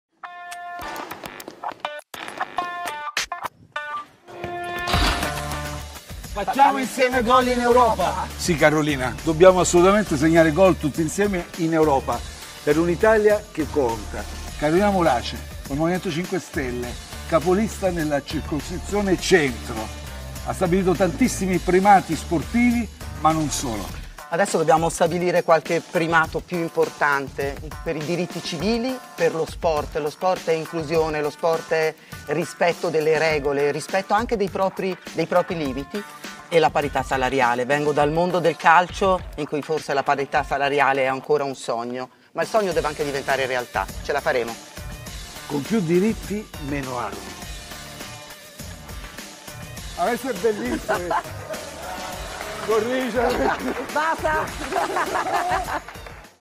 Conte e Morace giocano a calcio: "Facciamo goal in Europa"
La candidata: "Battaglia per diritti civili e parità salariale"

POLITICA (Roma). "Facciamo insieme goal in Europa! La battaglia per i diritti civili, la parità salariale, l'inclusività nello sport. Sono solo alcuni dei punti programmatici che porteremo in Europa con Carolina Morace, la nostra capolista nella circoscrizione Centro. Occorre la vostra partecipazione, serve un vostro assist per fare goal in Europa, per una 'Italia che conta'". Lo scrive sui suoi social il presidente del Movimento 5 Stelle Giuseppe Conte. Nel post viene pubblicato un video in cui il leader e la candidata alle europee nelle fila del M5s giocano a calcio in un cortile. Dopo l'assiste di Conte per il tiro di Morace, entrambi in favore di telecamera dicono: "facciamo insieme goal in Europa". (NPK) (Emilio Moretti/alanews)

facciamo insieme gol in Europa si Carolina dobbiamo assolutamente segnare gol tutti insieme in Europa per un'Italia che conta che avevamo l'acea è un Movimento 5 Stelle capolista nella circoscrizione centro ha stabilito tantissimi primati sportivi ma non solo adesso Dobbiamo stabilire qualche primato più importante per i diritti civili per lo sport e lo sport e inclusione lo sport rispetto delle regole rispetto anche dei propri dei propri limiti e la parità salariale vengo dal mondo del calcio in cui forse la parità salariale è ancora un sogno ma il sogno deve anche diventare realtà ce la faremo con più diritti meno alti